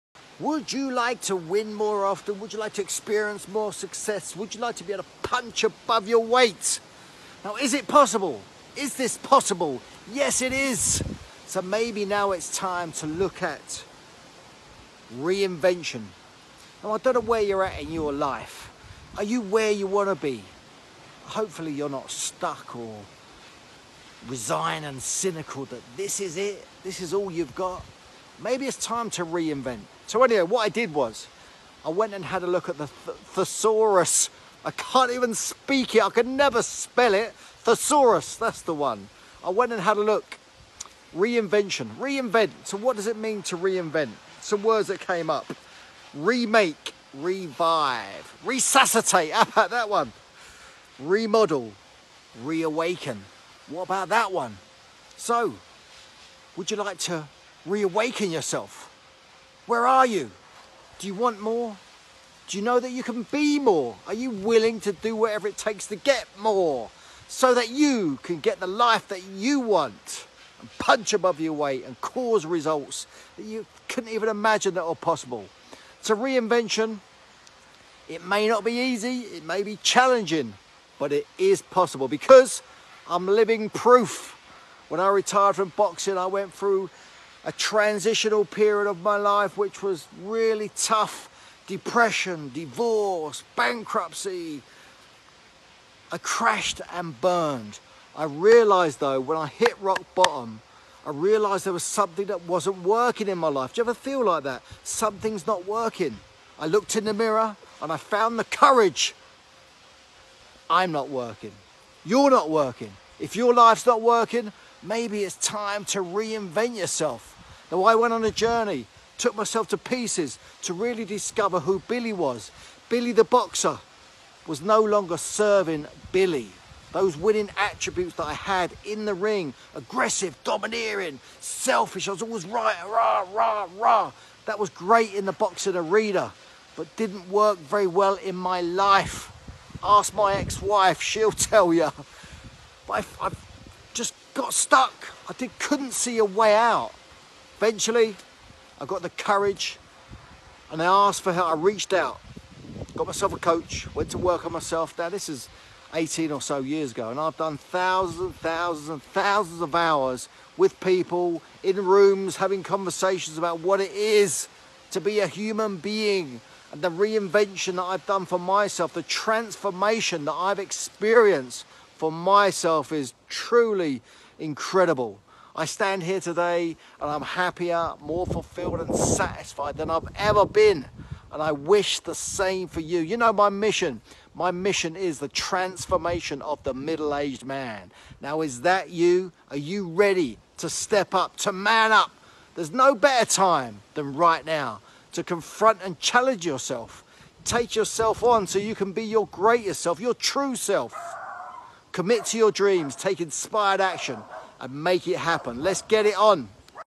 How to Reinvent yourself - Billy Schwer - Mental Boxing
To Men who want to Reinvent themselves.

Billy here!

Has the thought ever passed your mind…you know…to reinvent yourself?
I looked up the word reinvent in the Thesaurus (surprised myself there!)

Here’s some of what came up;
Remake
Revive
Resuscitate
Remodel
Renovate
Reawaken

If you had to pick one which currently applied to you…which one would it possibly be?

Now, there’s a BIG difference between wanting to Resuscitate your life and Remodel it.

And I know exactly how to do it…regardless of where you are NOW.
Check out todays tasty message for a short overview.

P.S. Book your complimentary RESUSCITATION 30 mins with me now, before you're too far gone!!